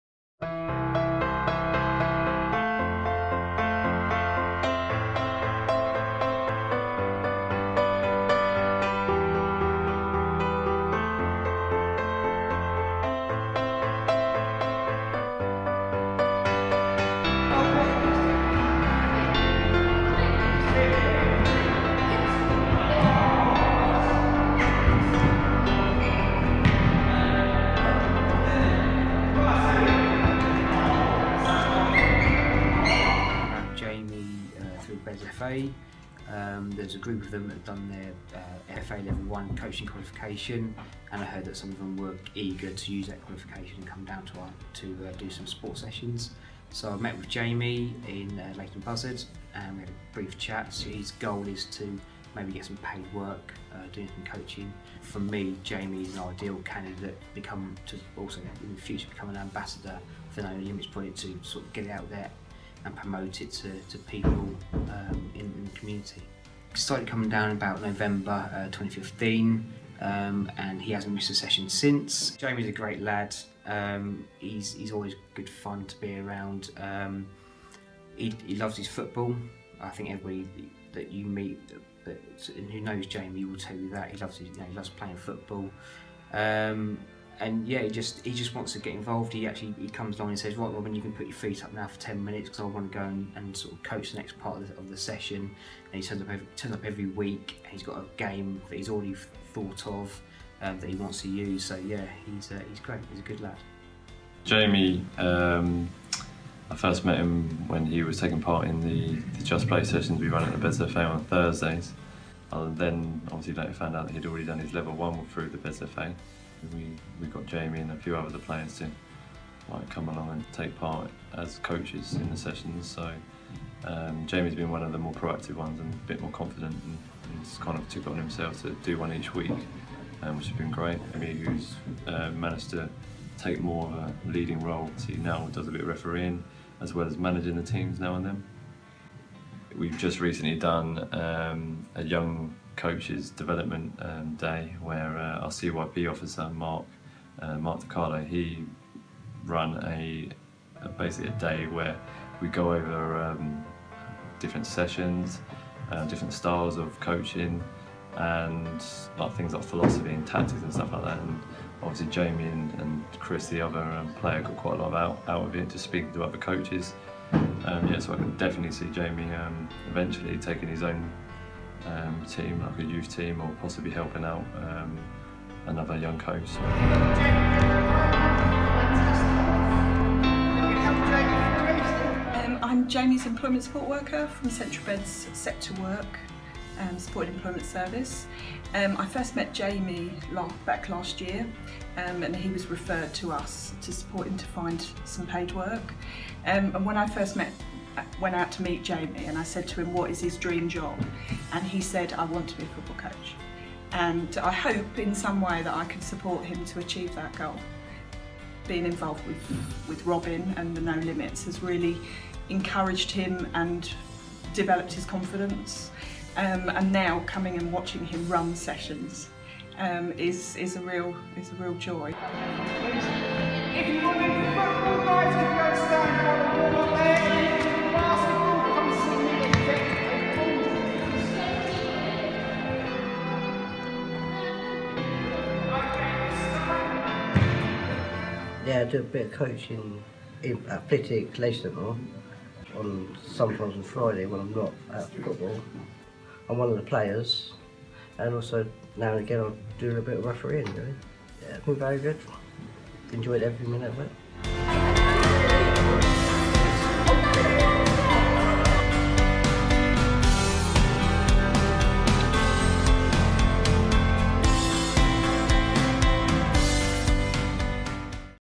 Central Bedfordshire Council No Limits Programme
A short video about Jamie Edwards, one of our No Limits Volunteers.

The No Limits programme offers people living with disability or health conditions access to exercise and fitness activities in Bedfordshire.